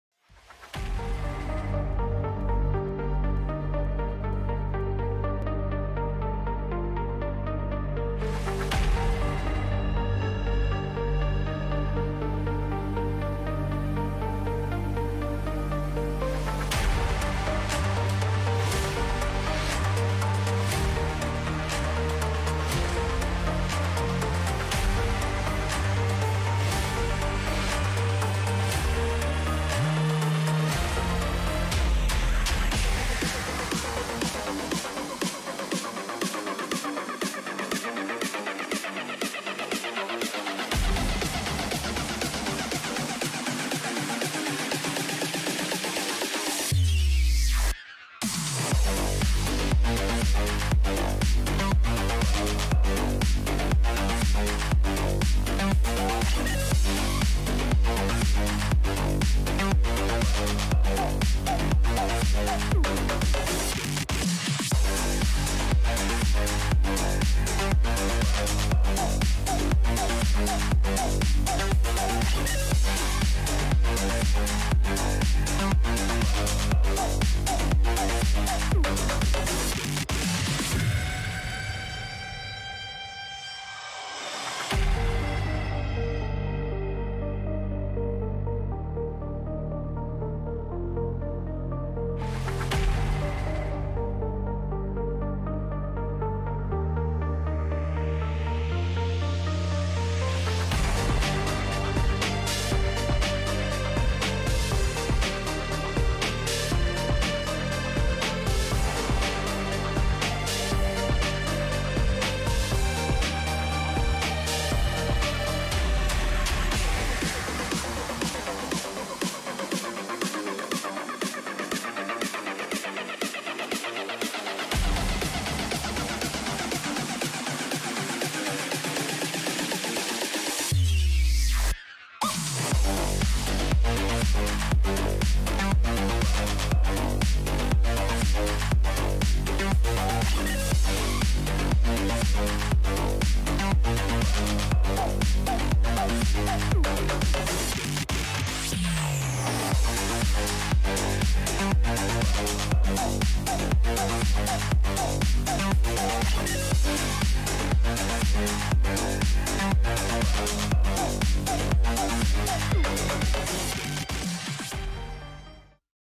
NHDP MCO 3.0 Home Skills Challenge Series - Boys & Girls U-14 (Week 4)
NHDP MCO 3.0 Home Skills Challenge Series - Boys & Girls U-14 (Week 4)Cabaran NHDP MCO 3.0 Home Skills Challenge Series bagi minggu keempat!

Minggu ini untuk kategori B-14 kami gabungkan peserta yang berjaya menunjukkan prestasi terbaik mereka.

Bagi minggu ini barisan jurulatih tidak memilih pemenang bagi cabaran Inverted V Test yang diberikan sebaliknya memilih peserta yang menunjukkan prestasi terbaik.

Nak tahu kenapa adik-adik ini dipilih sebagai peserta yang menunjukkan prestasi terbaik? Jom saksikan video yang telah disediakan.